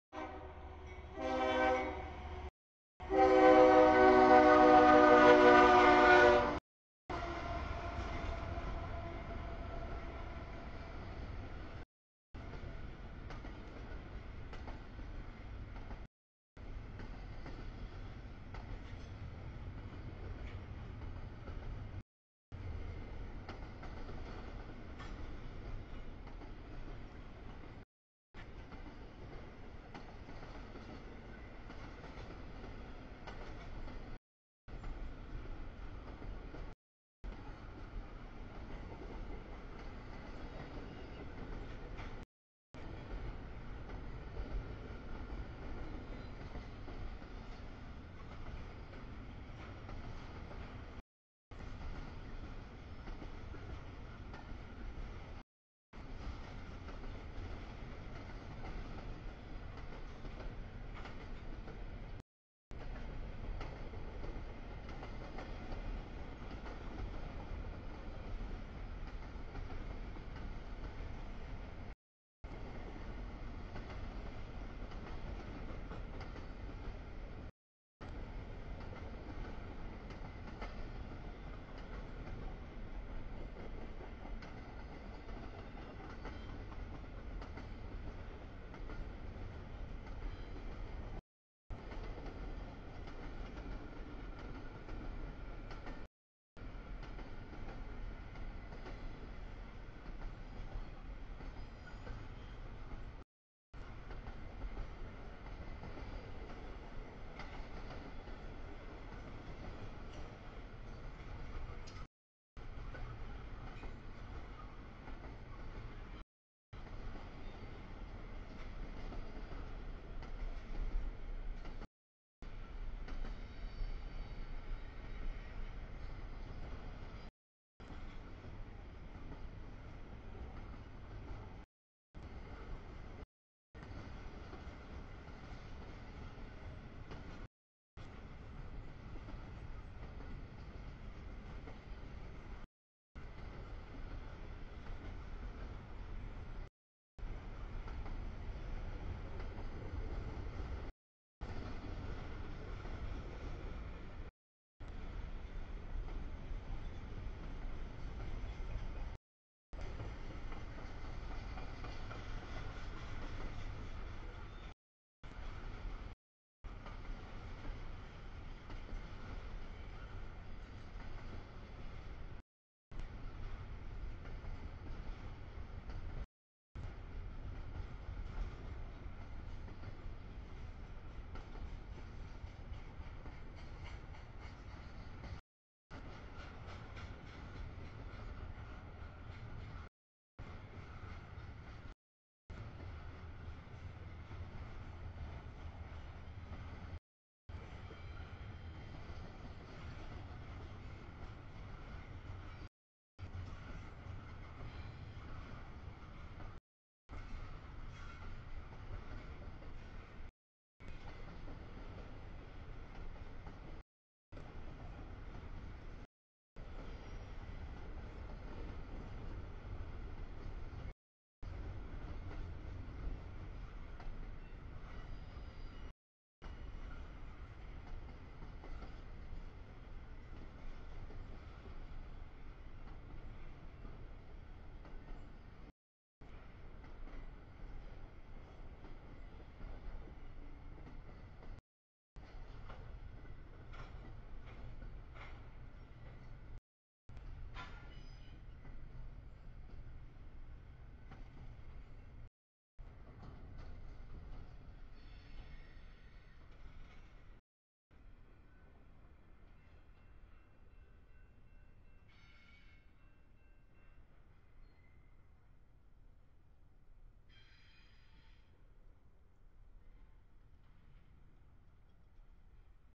2- 17-2019 21:05 sb stack CSX Q160
00:09 nb stack trailers CSX 3294 Q157
00:17 nb stack CSX 88 L159
00:34 nb stack trailers CSX 3342 Q009
02:36 nb mix, green trash CSX 739 DPU Q438
03:31 sb stack CSX 3130 Q162
03:44 nb auto rack CSX Q261
04:33 sb tanks BNSF BNSF CSX K162
06:19 sb stack trailers CSX Q010
07:18 nb stack trailers CSX 3456, 897 L003
08:54 sb stack trailers CSX 5448, 3269 Q008
09:59 sb stack CSX 3339, 5494 Q158
10:39 nb auto rack CSX 5238, 4566 Q253
12:04 nb mix CSX 580, 3001, 2768, 8145, 8094, 3270 Q190
12:57 nb mix, auto rack, nysw cut CSX 3392 mid DPU 955 Q410
13:50 sb green trash, mix CSX 3343, GECX 9406, CSX 6284, 2667 Q439
13:56 nb empty's triple BNSF 8389, 6866, 6813 K139
14:35 sb auto rack CSX 3390 Q262
15:14 sb mix epic/blue trash CSX 3322, 3014 Q433
15:24 nb mix, epic/blue trash CSX 5375, 719 Q434
16:55 sb auto rack CSX 5113, 8065 Q234
19:53 sb mix CSX 7390 mid DPU Q403
21:05 sb stack CSX Q160
21:40 sb stack CSX 851 DPU Q004
22:08 sb tanks triple BNSF K144
22:14 nb mix, green trash CSX 5415 Q438